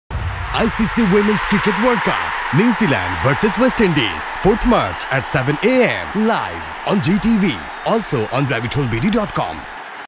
আইসিসি উইমেন্স ক্রিকেট ওয়ার্ল্ড কাপ - নিউজিল্যান্ড বনাম ওয়েস্ট ইন্ডিজ | Promo
আইসিসি উইমেন্স ক্রিকেট ওয়ার্ল্ড কাপ - নিউজিল্যান্ড বনাম ওয়েস্ট ইন্ডিজ - ICC WOMEN'S CRICKET WORLD CUP
দেখবেন ৪ মার্চ, সকাল ৭টায়